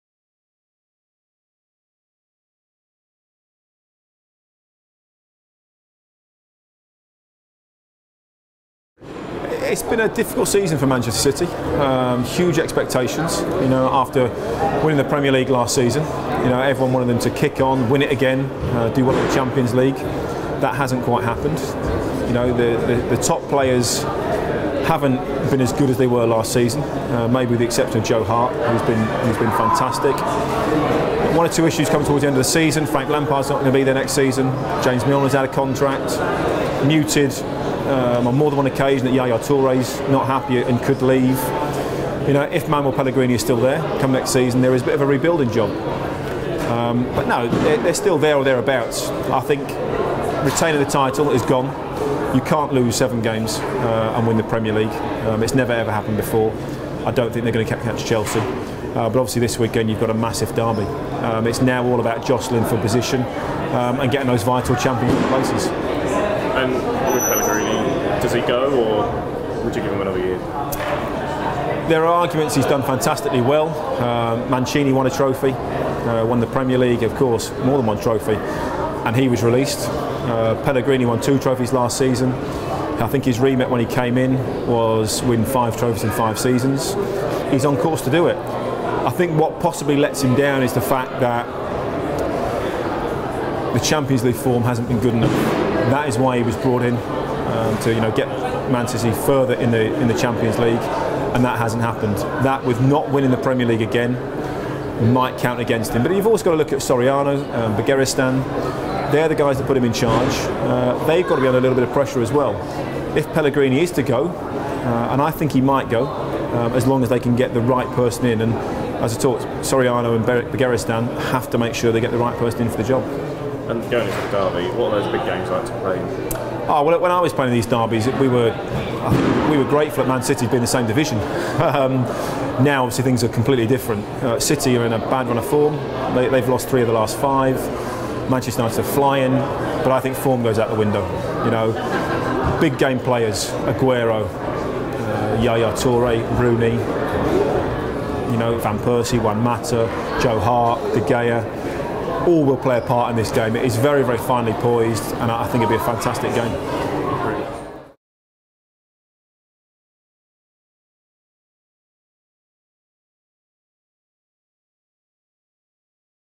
WATCH: Danny Mills talks Manchester City, Pellegrini and derby day.
Former Manchester City and England player Danny Mills talks to Lewis Smith about City's season, Manuel Pellegrini's future and the upcoming Manchester derby.